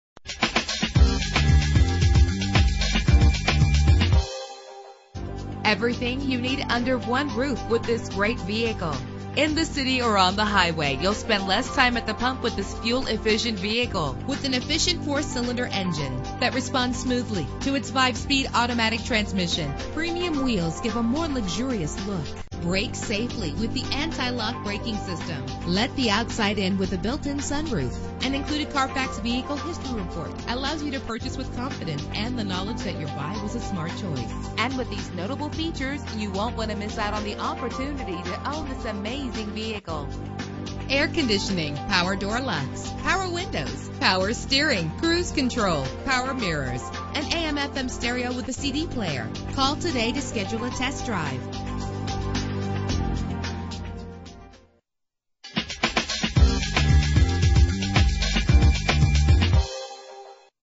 1998 Mercedes-Benz C-Class - Bothell WA
Year: 1998
Make: Mercedes-Benz
Model: C-Class
Trim: C230
Engine: 2.3 liter inline 4 cylinder 16 valve
Transmission: 5-Speed Automatic
Color: White
Mileage: 180065
Address:
17318 Bothell Way NE
Bothell, WA 98011